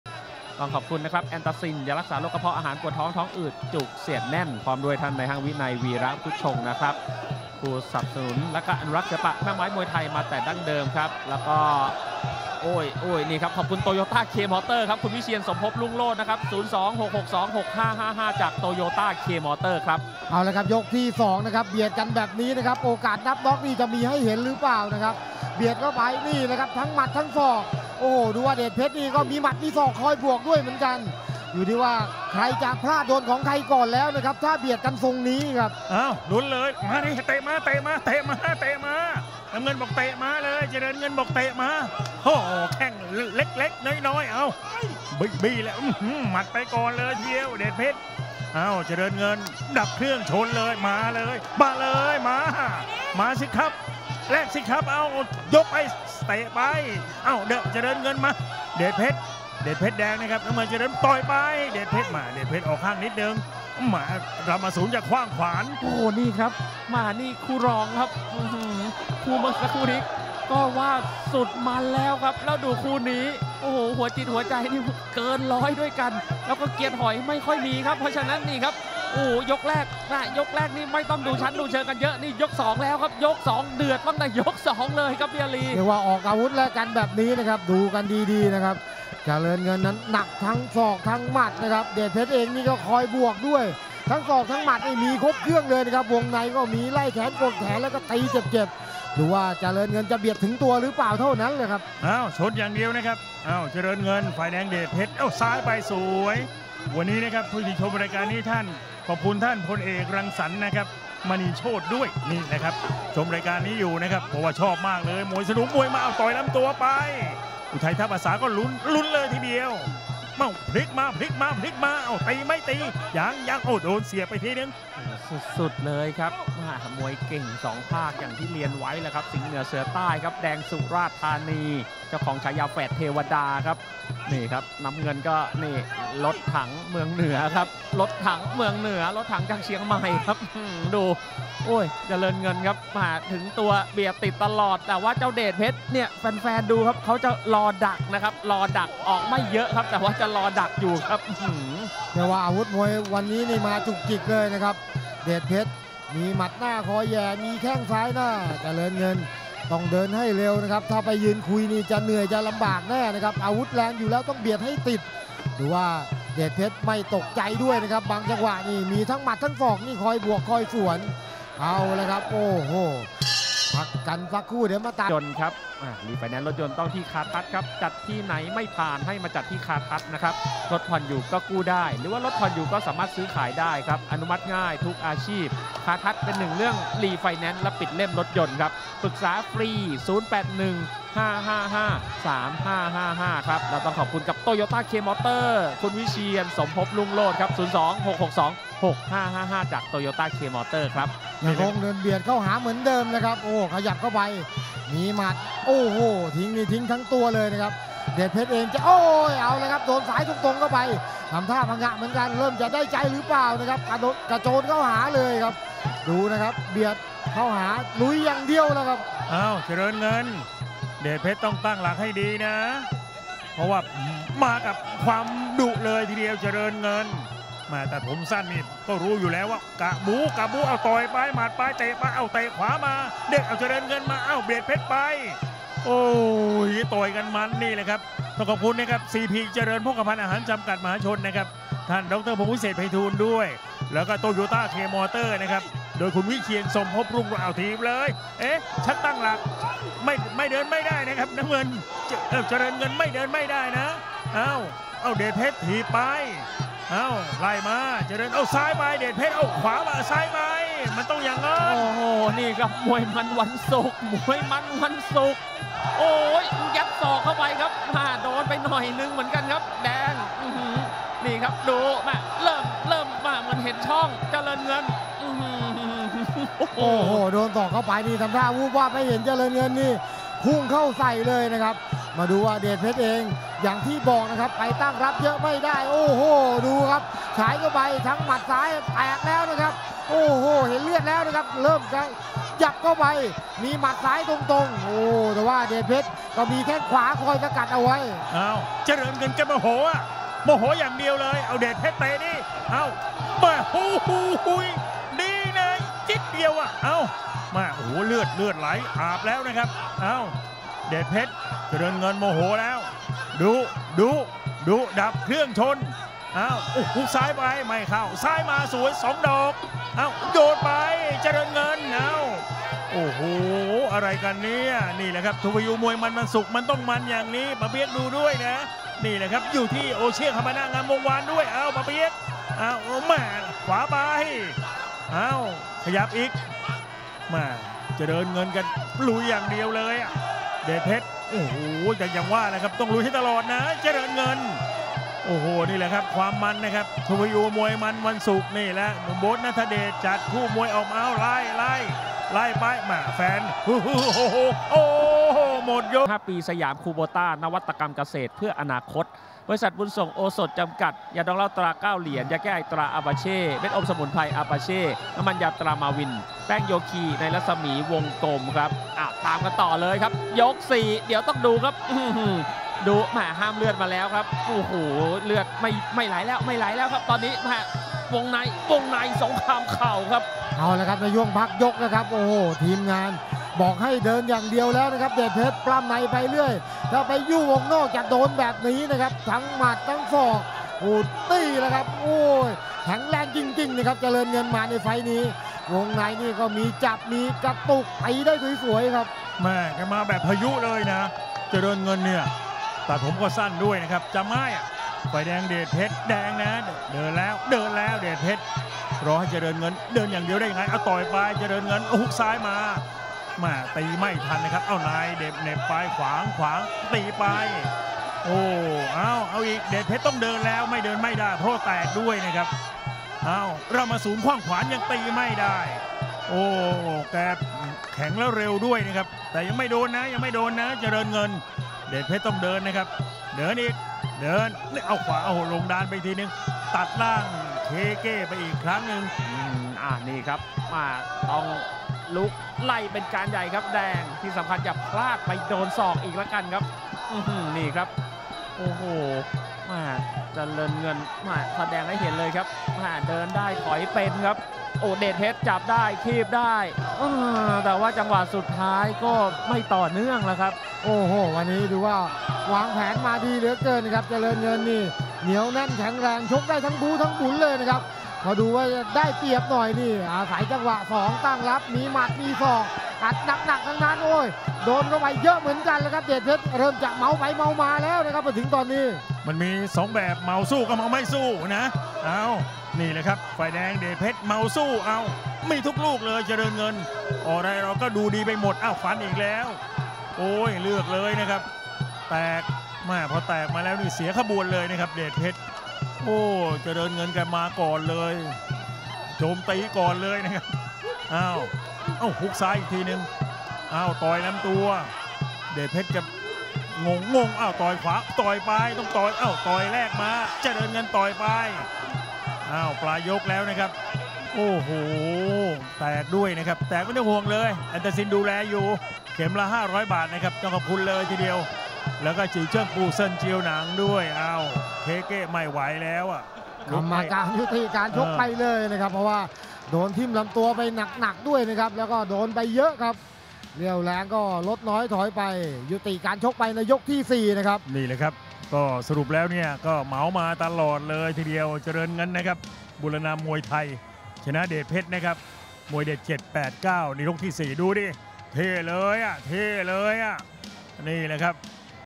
เดชเพชร VS เจริญเงิน | DEJPETCH VS JARERNNGERN | มวยมันส์วันศุกร์
เดชเพชร VS เจริญเงิน | DEJPETCH VS JARERNNGERN | มวยมันส์วันศุกร์ วันศุกร์ ที่ 5 พฤษภาคม 2566 เดชเพชร มวยเด็ด789 VS เจริญเงิน บุญลานนามวยไทย Suek MUAYMUNSANANMUEANG  Fight between DEJPETCH MUAYDED789 VS JARERNNGERN BOONLANNAMUAYTHAI 5 May, 2023 MUAYMUNWUNSUK , Rangsit Stadium